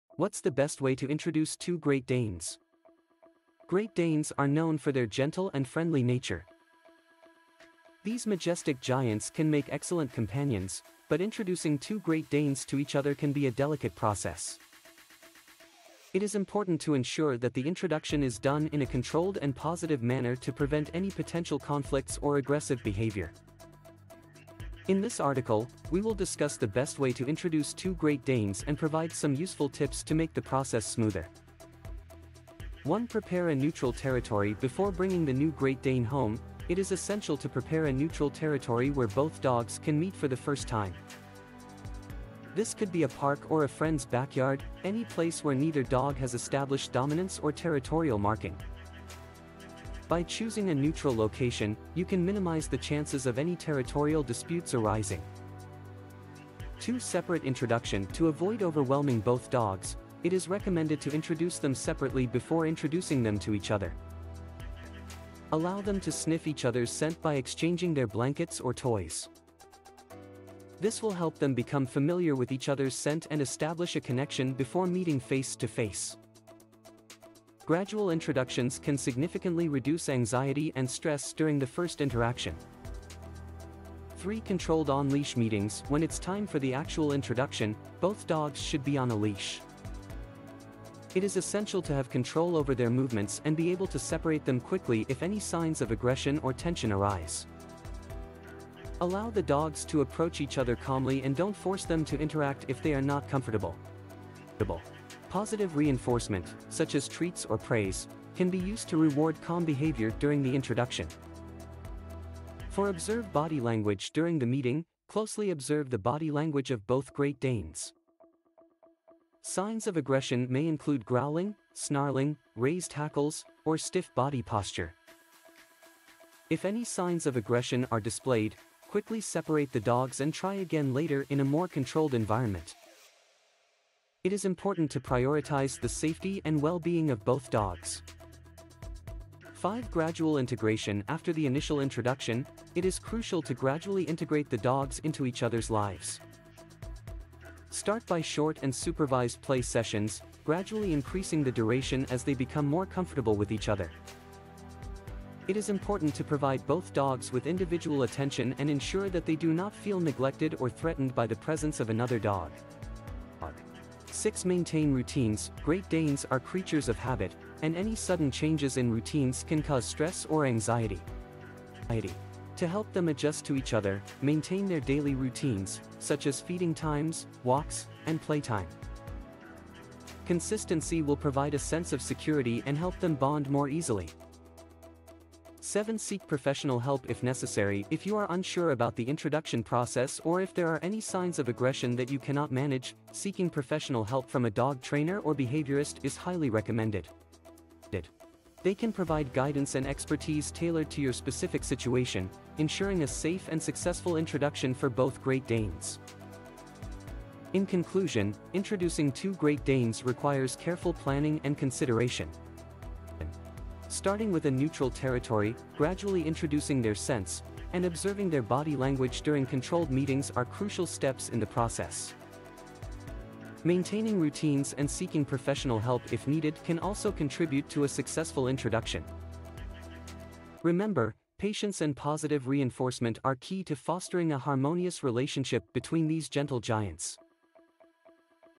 What s the Best Way to Introduce Two Great Danes?
Great Danes are known for their gentle and friendly nature  These majestic giants can make excellent companions, but introducing two Great Danes to each other can be a delicate process  It is important to ensure that the introduction is done in a controlled and positive manner to prevent any potential conflicts or aggressive behavior  In this article, we will discuss the best way to introduce two Great Danes and provide some useful tips to make the process smoother

1 Prepare a neutral territory:
Before bringing the new Great Dane home, it is essential to prepare a neutral territory where both dogs can meet for the first time
00:00 What's the Best Way to Introduce Two Great Danes?